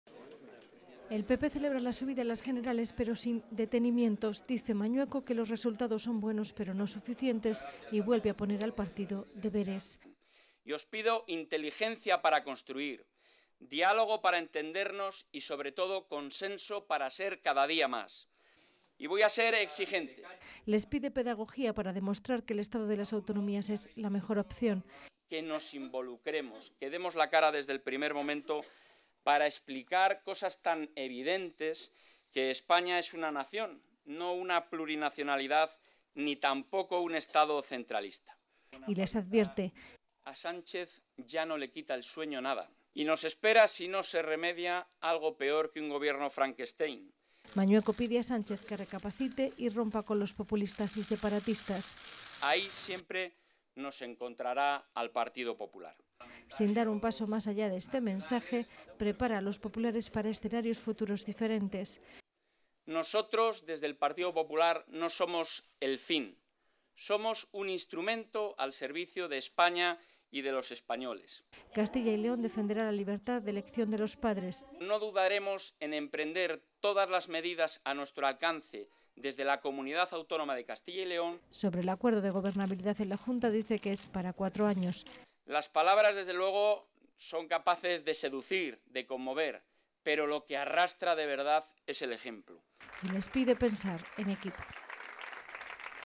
Mañueco pide que Sánchez rompa con los 'populistas y separatistas' para recomponer los consensos
El presidente del PPCyL critica que el presidente en funciones haya pasado del 'no me vale nada a me vale cualquier cosa'